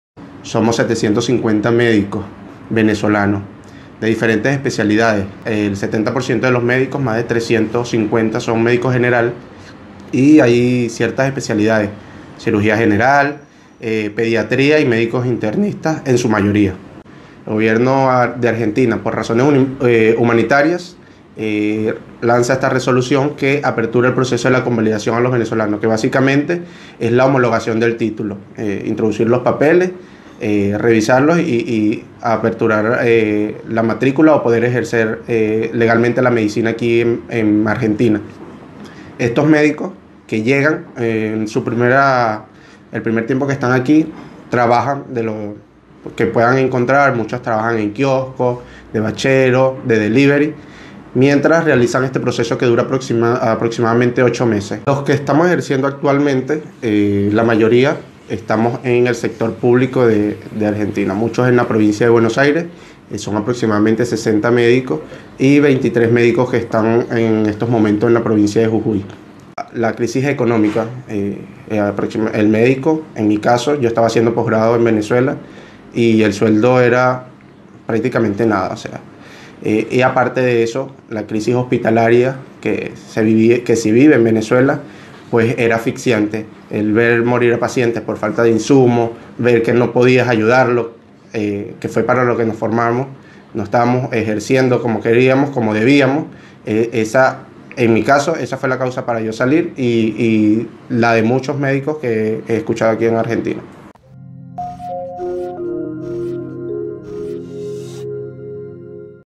750 médicos venezolanos se encuentran en la espera de la convalidación de títulos en Argentina
Son 750 médicos venezolanos en Argentina de diferentes especialidades que se encuentran en la espera de la convalidación de títulos que dura 8 meses para poder ejercer su carrera. Mientras los trámites están en regla muchos tienes que realizar oficios en diferentes áreas para poder subsistir como inmigrantes.